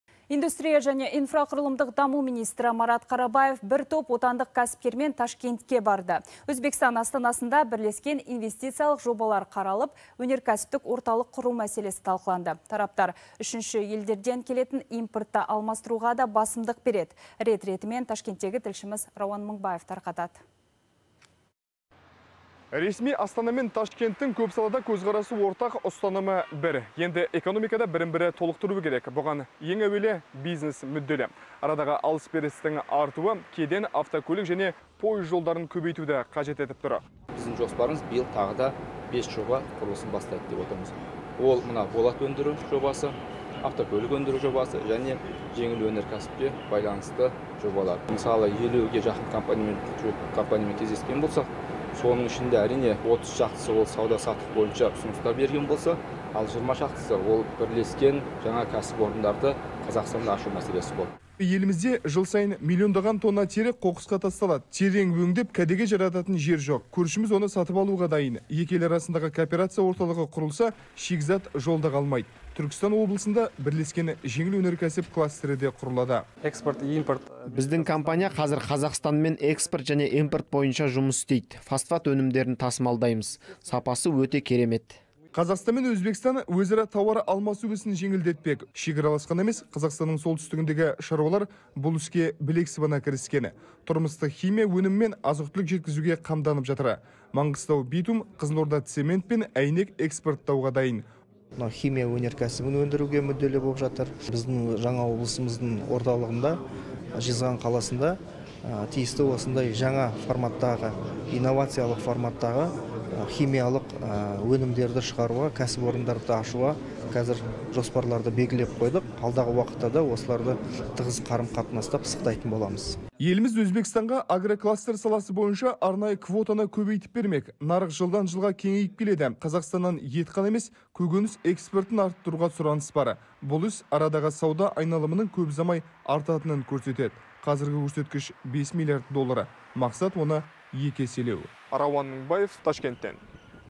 Марат Қарабаев бір топ отандық кәсіпкермен Ташкентке барды
Өзбекстан астанасында бірлескен инвестициялық жобалар қаралып, өнеркәсіптік орталық құру мәселесі талқыланды.